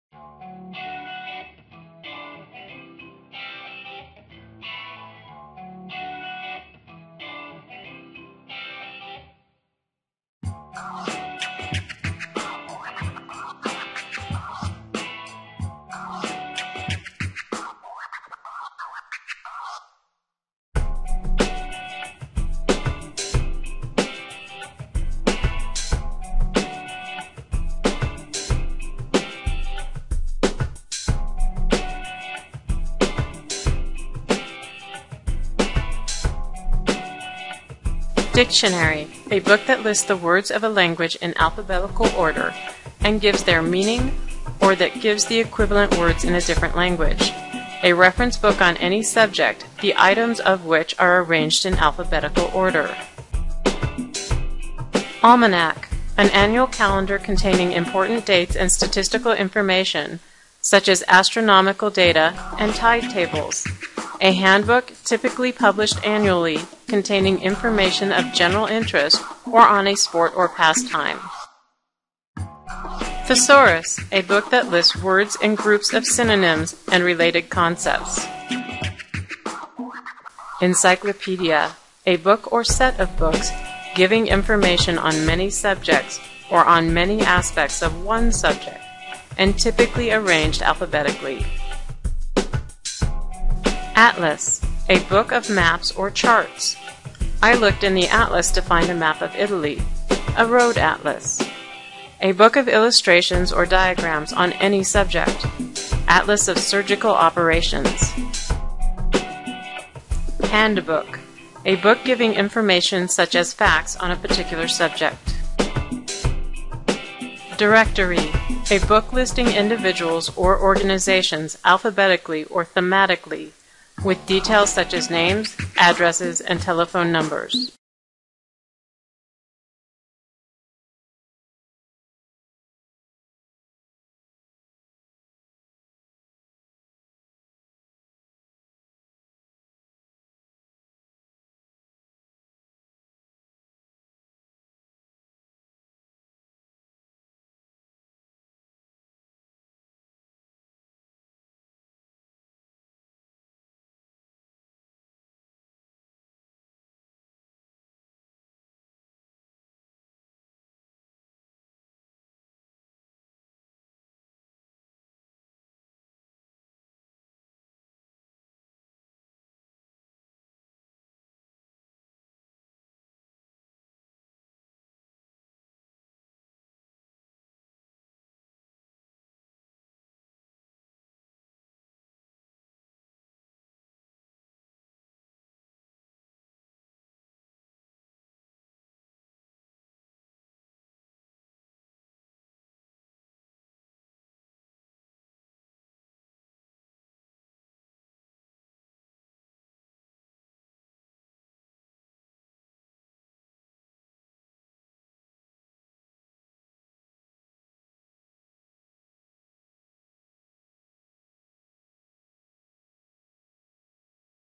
Reference Sources Part One
This video gives information on online, full text reference sources at the University of Northern Colorado Libraries.